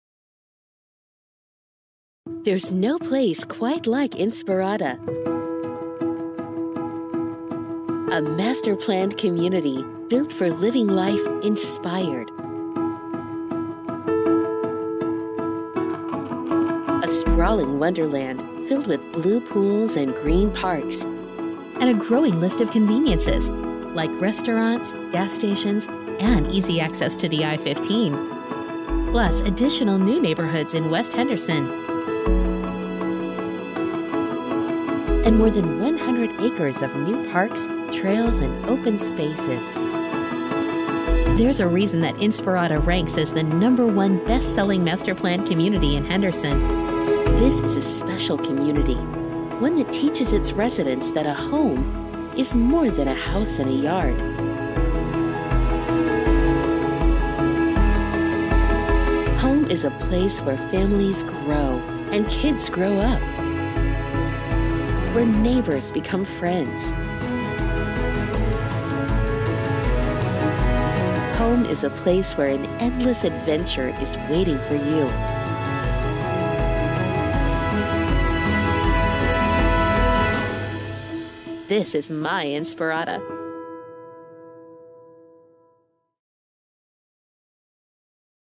Toll Brothers at Inspirada
A drone video of the communities at Inspirada. ;